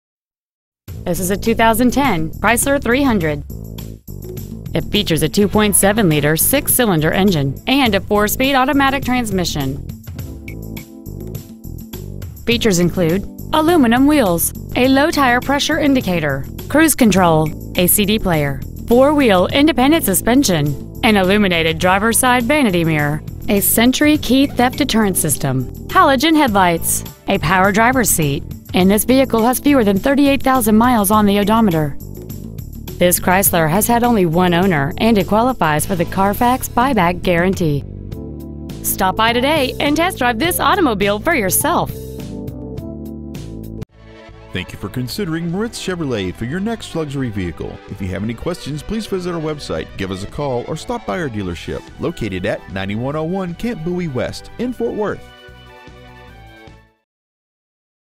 Preowned 2010 Chrysler 300 Arlington TX
Year : 2010
 Make : Chrysler
 Model : 300
 Engine : Gas V6 2.7L/167
 Trans . : Automatic
 Exterior : Gold
 Miles : 37,356
 Interior : Dark Slate Gray
 Stock : P17051